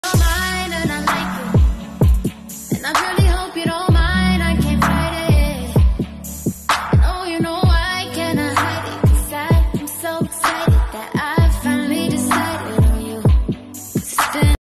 Double The Bow.. Double the Fun!
Handmade bows that can be paired with headband, hairclips, or elastic band for bowtie! lets accessorize the littles!